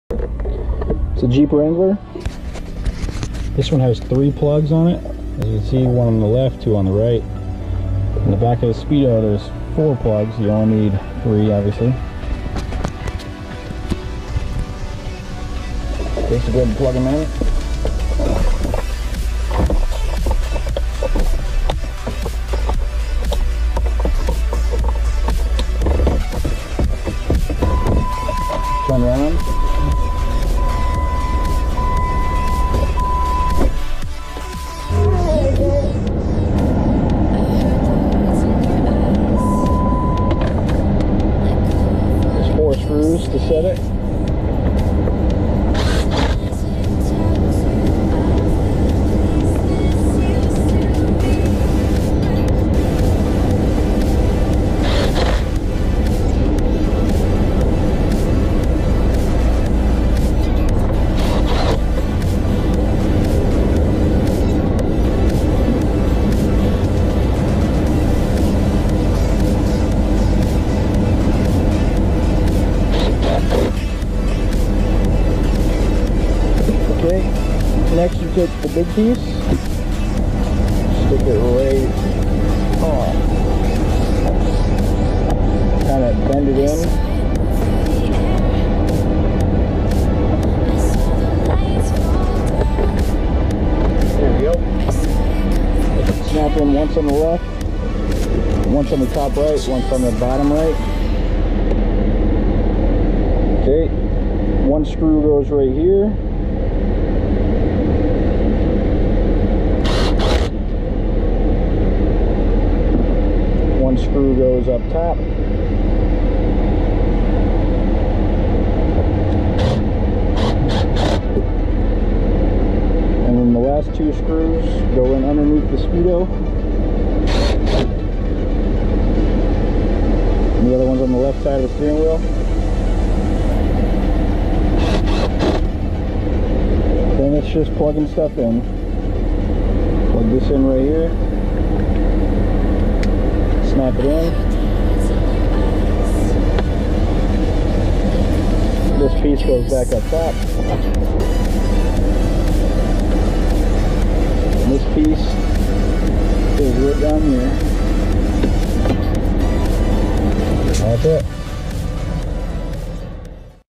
How To Put Speedo In Jeep Wrangler
How To Video On How To Put The Speedometer/Dashboard Back Together In A Jeep Wrangler.